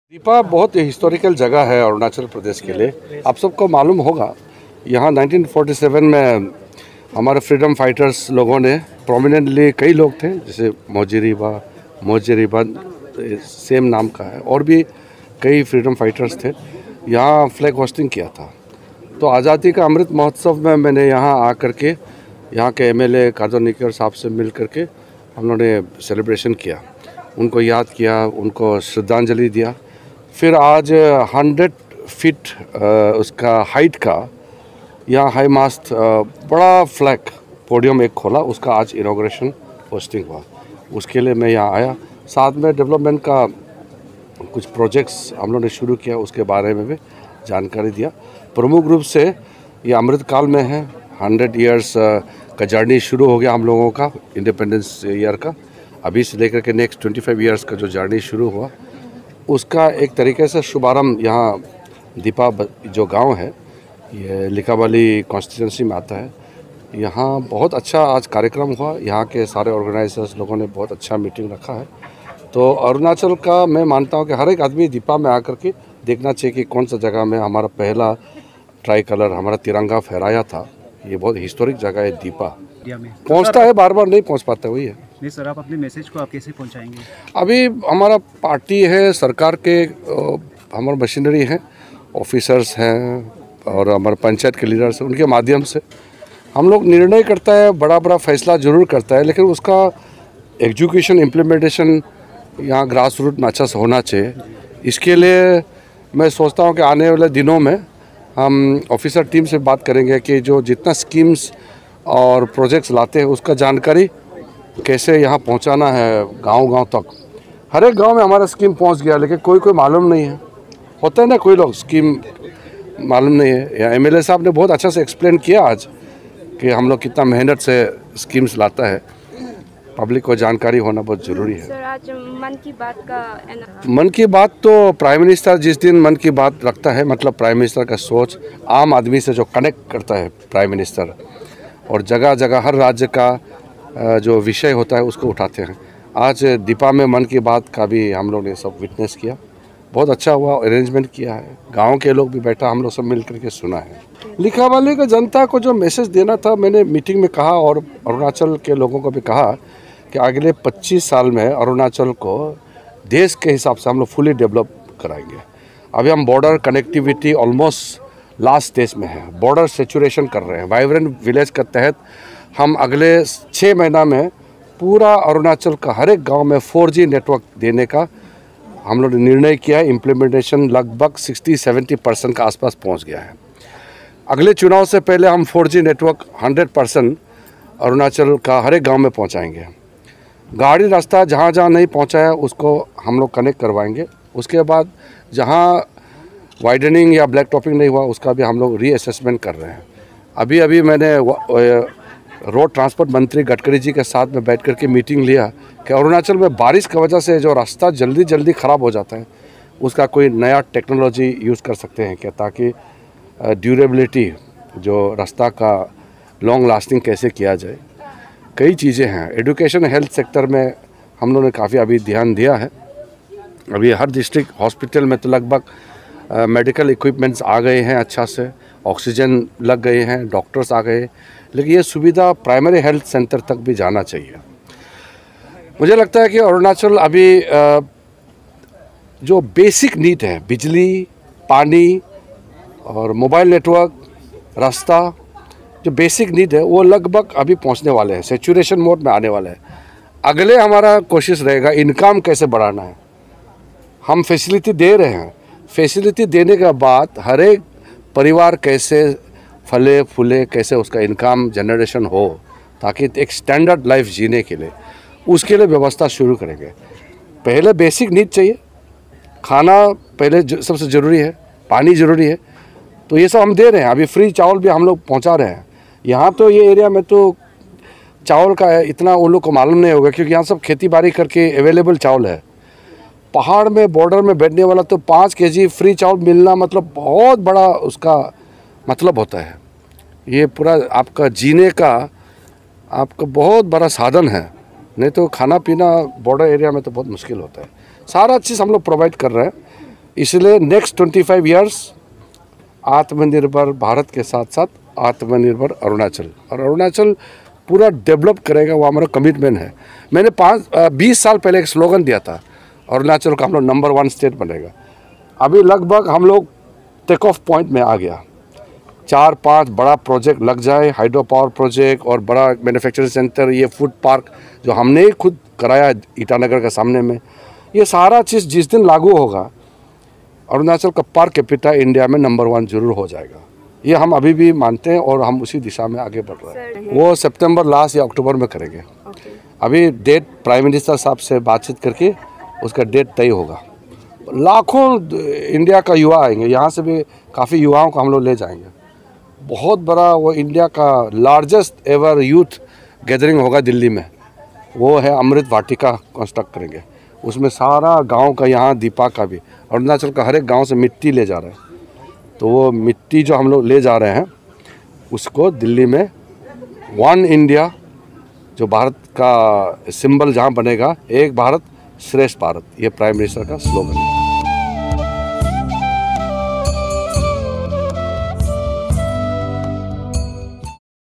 অৰুণাচল প্ৰদেশৰ লিকাবালিৰ উন্নয়ণৰ খবৰ ল'বলৈ গ'ল কেন্দ্ৰীয় মন্ত্ৰী কিৰণ ৰিজিজু।
১০০ ফুট ওখ ৰাষ্ট্ৰীয় ধ্বজা উত্তোলন।

স্বাধীনতা যুঁজাৰুৰ প্ৰতি সশ্ৰদ্ধ সোঁৱৰণ।‌